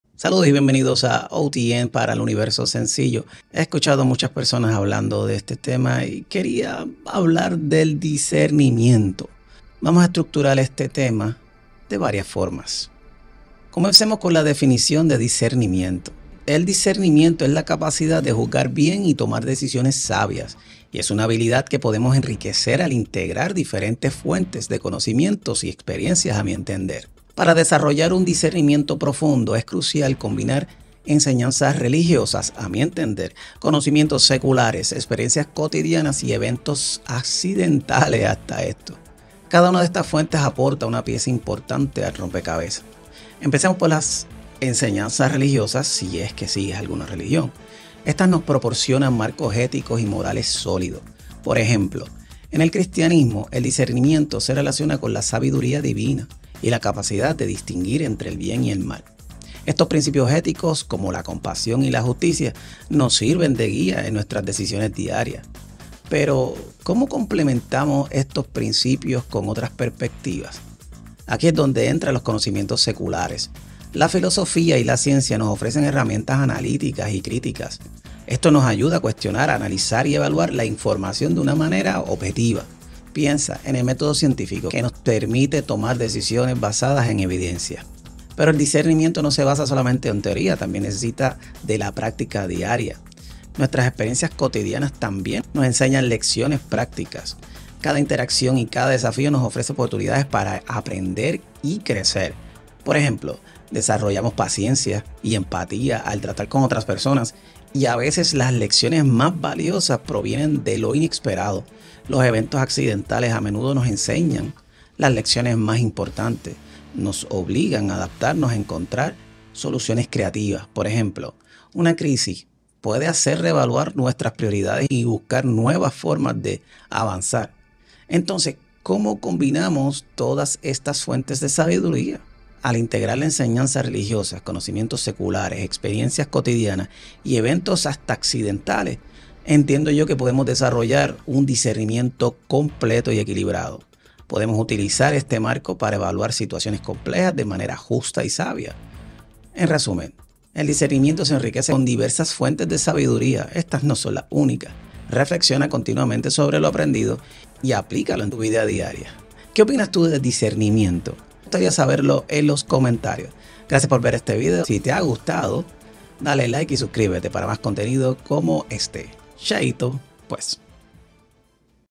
¿El Discernimiento que es?
El discernimiento es la capacidad de tomar decisiones sabias, enriquecido por enseñanzas religiosas, conocimientos seculares, experiencias cotidianas y eventos accidentales. Al integrar estas fuentes, desarrollamos un juicio equilibrado para evaluar situaciones complejas de manera justa y sabia